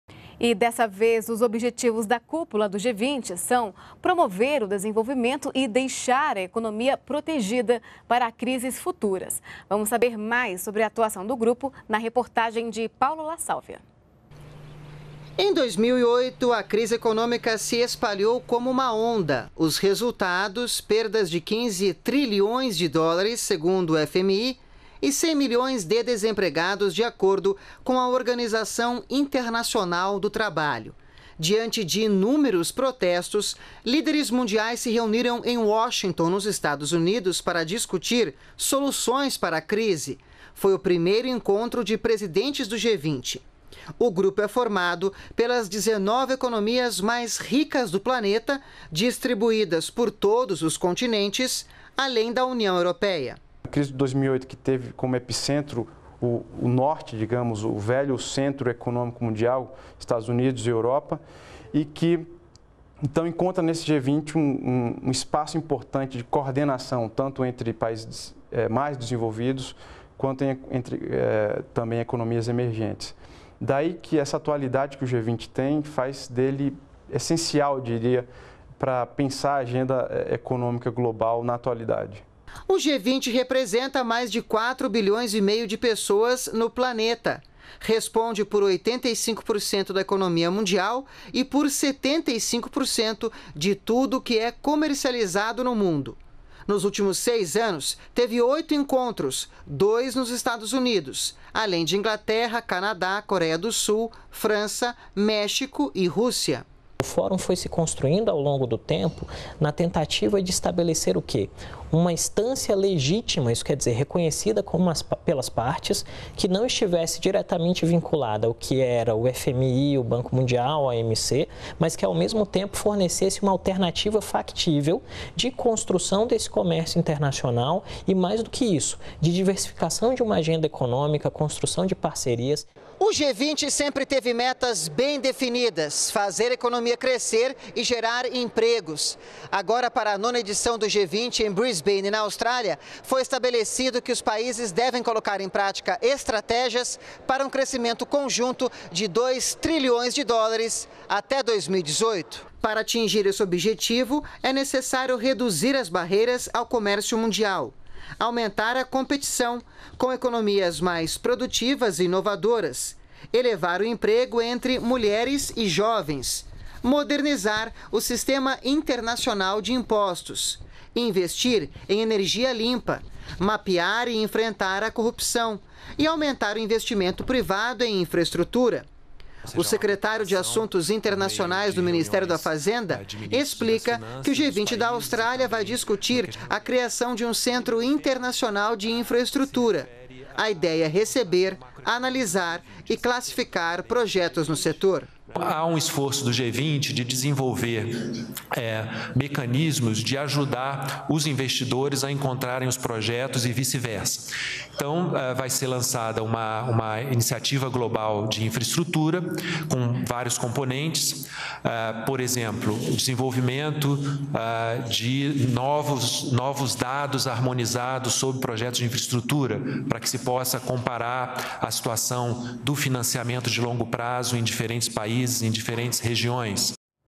Nona cúpula do G20 começa neste sábado em Brisbane na Austrália
NBR NOTÍCIAS - 14.11.14: A 9ª Cúpula do G20 começa, neste fim de semana, em Brisbane, na Austrália. O tema deste ano é o crescimento econômico global e a geração de empregos. O grupo é formado pelas 19 economias mais ricas do mundo e da União Européia. Os chefes de Estado e Governo começaram a participar do encontro em 2008, com o agravamento da crise econômica mundial. O G20 representa mais de 4,6 bilhões de pessoas e responde por 85% da economia mundial. Nos últimos seis anos, foram realizados oito encontros, dois nos EUA, além de Inglaterra, Canadá, Coreia do Sul, França, México e Rússia.